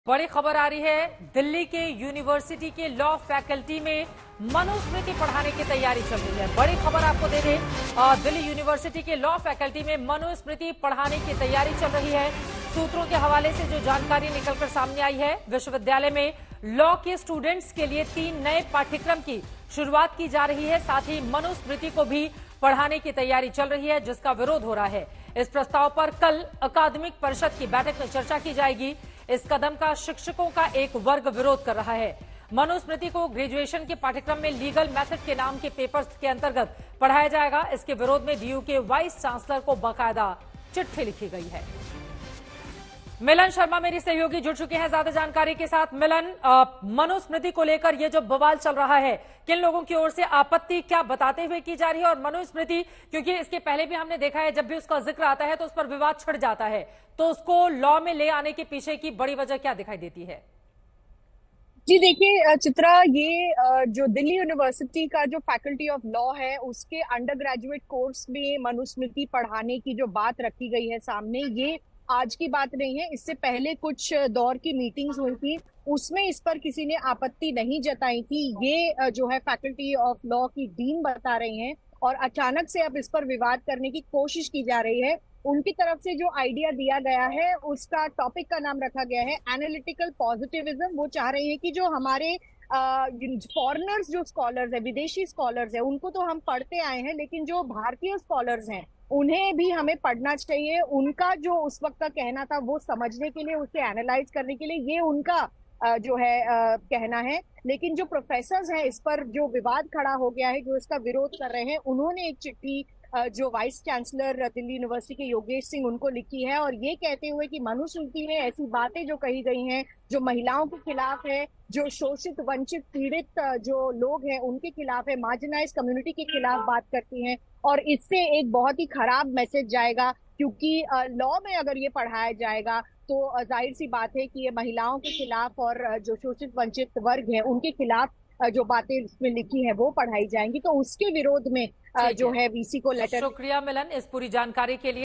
Breaking News: Delhi University में लॉ स्टूडेंट्स को मनुस्मृति पढ़ाने की तैयारी | Manusmriti
दिल्ली यूनिवर्सिटी के लॉ फैकल्टी में मनुस्मृति पढ़ाने की तैयारी चल रही है, सूत्रों के हवाले खबर है कि दिल्ली विश्वविद्यालय में लॉ के स्टूडेंट्स के लिए तीन नए पाठ्यक्रम की शुरुआत की जा रही है और साथ ही मनुस्मृति को भी पढ़ाने की तैयारी चल रही है जिसका विरोध हो रहा है...इस प्रस्ताव पर कल अकादमिक परिषद की बैठक में चर्चा की जाएगी, इस कदम का शिक्षकों का एक वर्ग विरोध कर रहा है...मनुस्मृति को ग्रैजुएशन के पाठ्यक्रम में लीगल मेथड नाम के पेपर के अंतर्गत पढ़ाया जाएगा. इसके विरोध में डीयू के वाइस चांसलर को चिट्ठी लिखी गई है...